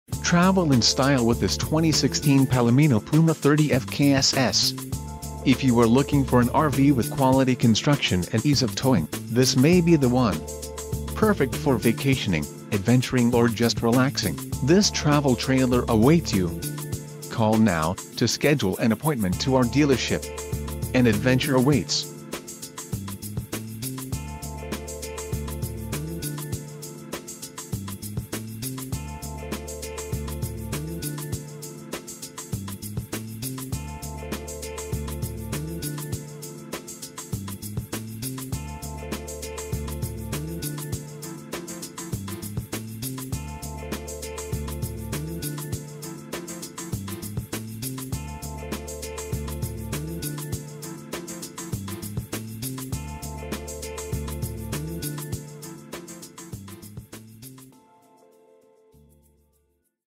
2016 Palomino Puma 30FKSS - Colonia Del Rey RV - Corpus ...
Options: Back up Camera Ready, Pre-Wired for Solar System, LED Lighting, 13,500 BTU Ducted A/C, 6cf Double Door Refrigerator, Wood Grain Linoleum, Bed Spread, Bedroom TV Hook Up, TV Antenna w/Booster and Cable Hook Up, Mini Blinds, Tub Surround One Piece Fiberglass, AM/FM CD/DVD Stereo w/Sound Bar, Range Cover (Metal), Under Mount Sink w/Cover, Electric Awning, Upgrade Colored Metal, Diamond Plate, Push Back Recliner(s) (where available), Glass Shower Door (where available), Over Head Cabinets in Super Slide Room, Carbon Monoxide Detector, Trifold Sofa IPO Jack Knife in Living Area, 15,000 BTU Ducted A/C IPO 13,500 BTU, Oven, Bath Skylight, Expresso Raised Panel Cabinet Doors w/Frig Insert, Folding Grab Handle, 50 Amp Service (2nd air), 30 lbs Gas Bottles and Cover, 4 Stabilizer Jacks, Enclosed Underbelly, 2 Outside Speakers, 6 Gallon Gas/Electric Hot Water Heater, Outside Shower, Night Shades, Spare Tir... read more